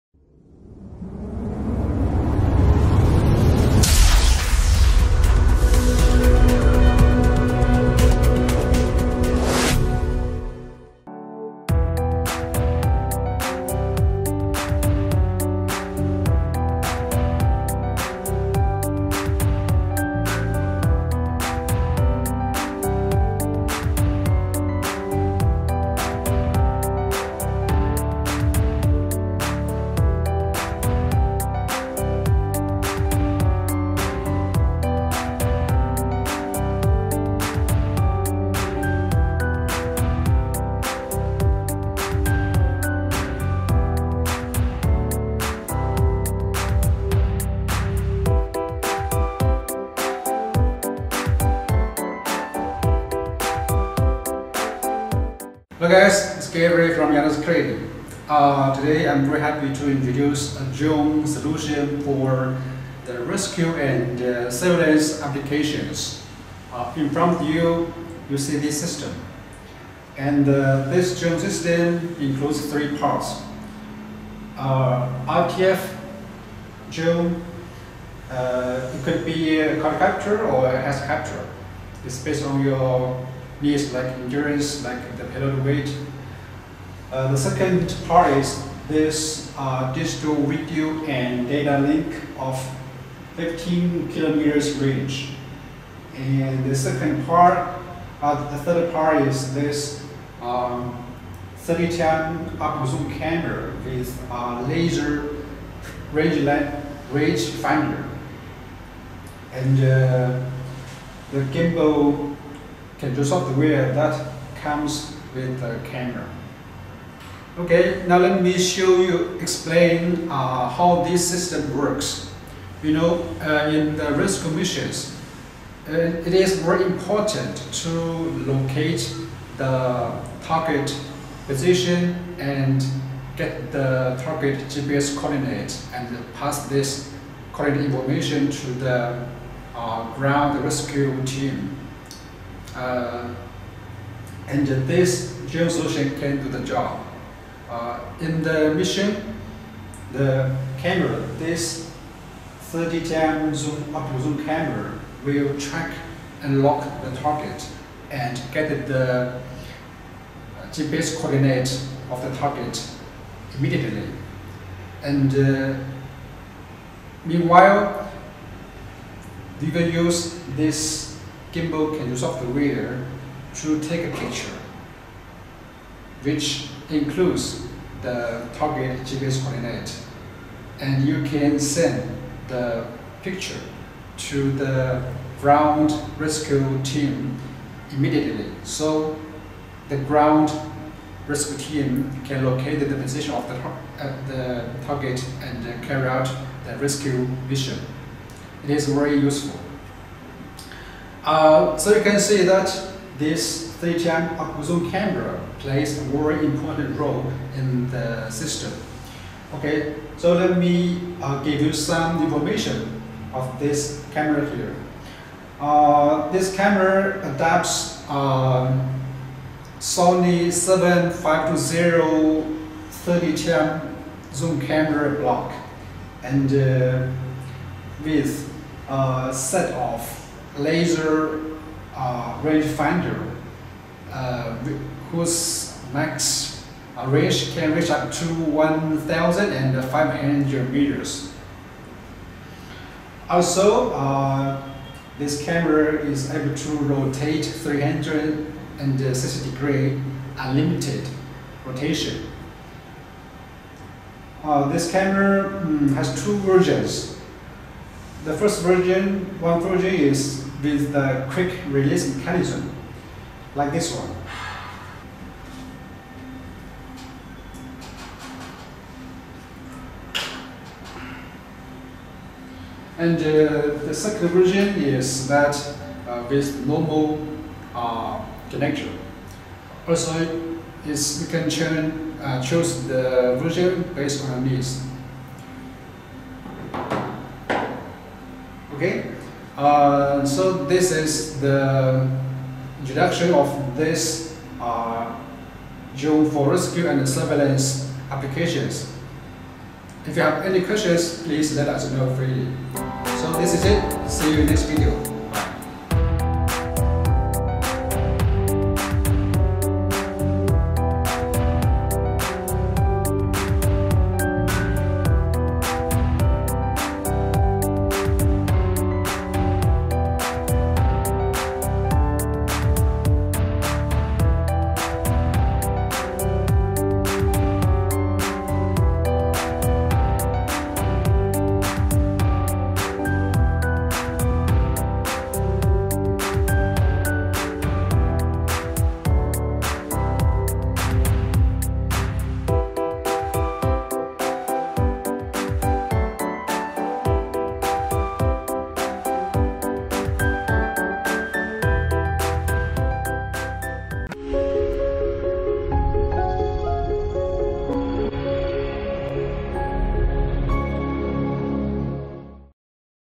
YANGDA 30X Optical Drone Zoom Camera With Tracking And Target GPS
When the drone is conducting a rescue mission, the most important thing is to locate the target position ASAP, get the exact GPS coordinate, and send the coordinate information to the ground rescue team immediately. And this SAR drone system can do the job.

About the 30x zoom camera Sky Eye-30HZ-SM
Sky Eye-30HZ-SM is a 3-axis high stabilized gimbal with Full HD 1080P 30X optical zoom camera and a set of laser rangefinder, which could be wildly used for surveillance, search and rescue applications. Its core capability is able to get the target GPS coordinate and measure the distance between the gimbal and the target. Together with the gimbal control software that comes with gimbal, the 30X drone zoom camera will enable the ground rescue team to locate the target as soon as possible.

Sky Eye-30HZ-SM weighs as little as 587g to help you meet your payload weight allowance.

High-quality sensors
The drone gimbal adopts SONY FCB-EV7520 30X optical zoom camera, which provides 1080P 60FPS full HD video streaming and up to 360X zoom capability, which will enable you to see every detail you need in the air even you are far away from the object.
Also, the max range of the laser rangefinder can reach up to 1500 meters.

Gimbal quick-release mechanism
The 30X zoom camera comes with a quick-release mechanism, provides Ethernet video streaming output, and 360-degree unlimited rotation.

Auto object tracking
With the on-board object tracking module, Sky Eye-30HZ-SM can track static or moving targets smoothly, keeping it in the center of the frame.

2 modes for video out and video recorded
Mode 1: 1080P/30fps video out, and 720P/30fps video recorded.
Mode 2: 720P/30fps video out, and 1080P/30fps video recorded.